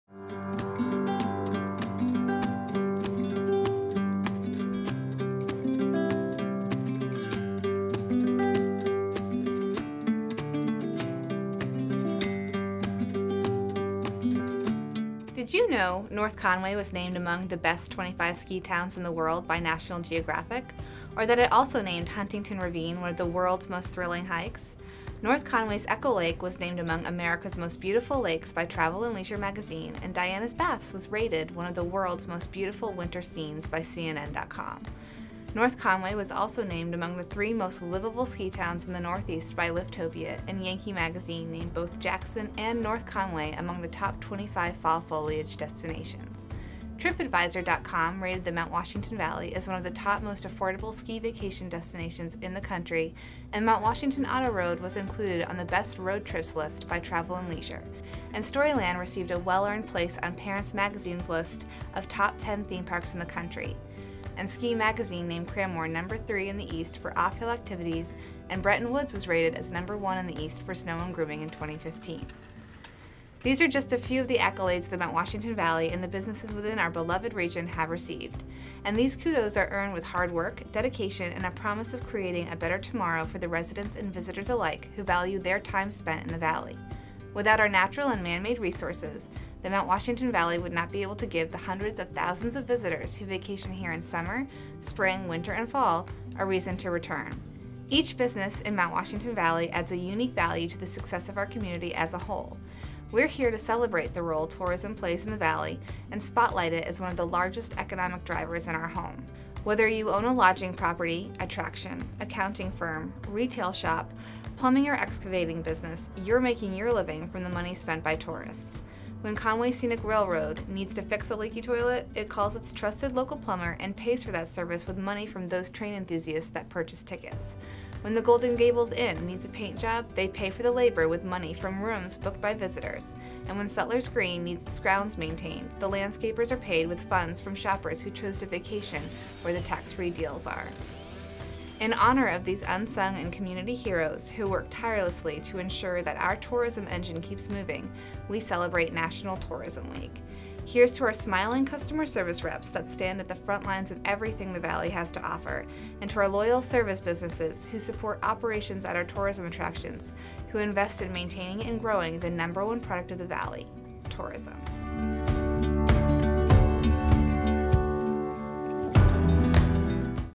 Mt. Washington Valley Celebrates Tourism
In recognition for the role tourism plays in the Mt Washington Valley economy and in conjunction with National Tourism Week, Mt Washington Valley Chamber of Commerce (MWVCC) is initiating a new event and award program in 2015.  Celebrating and spotlighting tourism,  the largest economic driver in the Mt Washington Valley, this event will honor the unsung and local heroes within our community and bring a focus to the part that tourism plays in the Mt Washington Valley economy.  This year’s theme for the Tourism Rally focuses on the service sector and how tourism dollars drive the economic engine in the Valley, for those professions directly involved in the front lines and the businesses and people who support tourism.